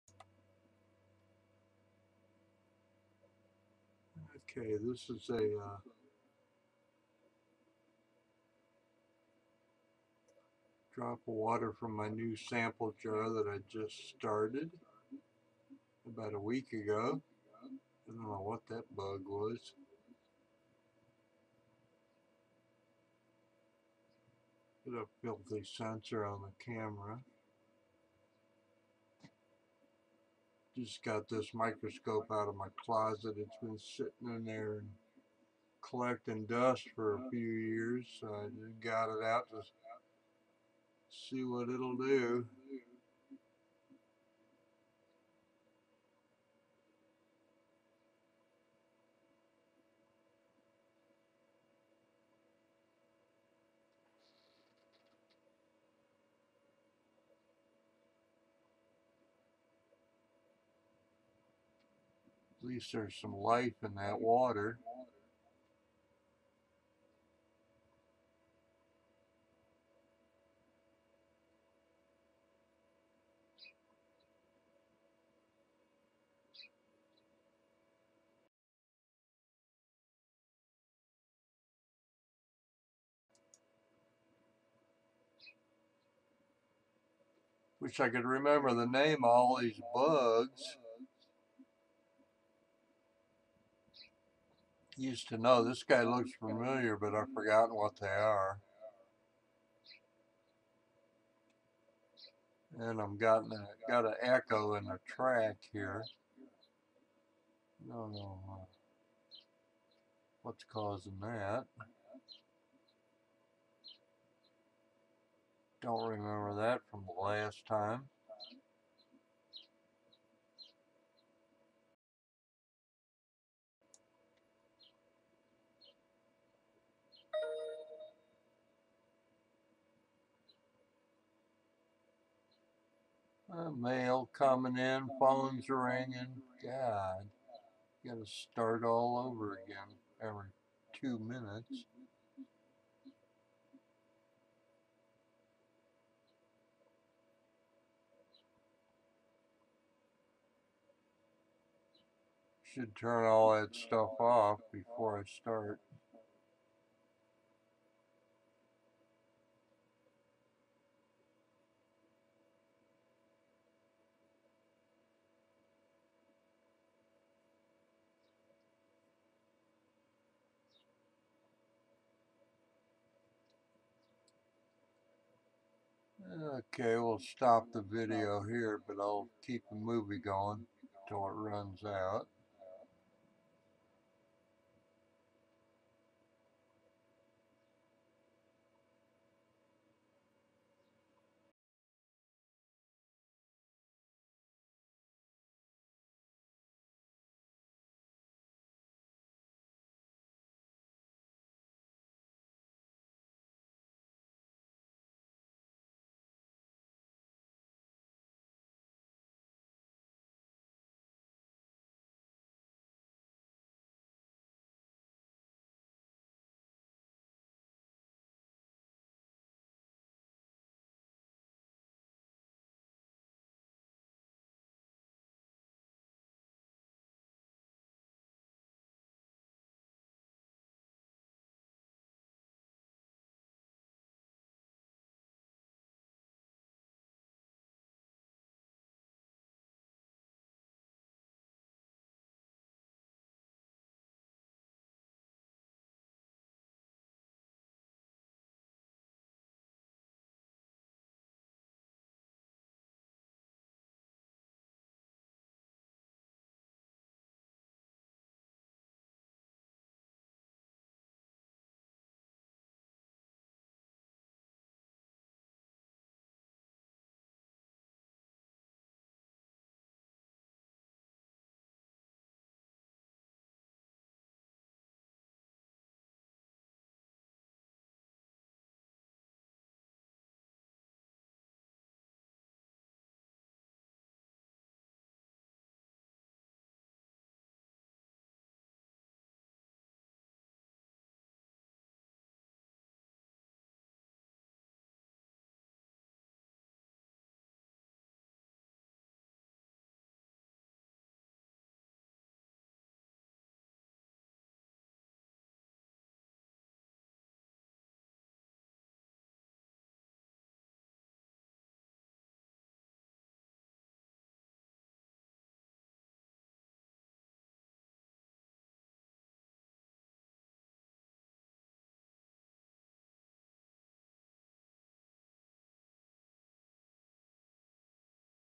New Life in my Sample Jar
Finally seeing some life, but have forgotten all the names of these bugs after 3-4 years of not using the scope. It'll come back, but if you know, please tell me. LOL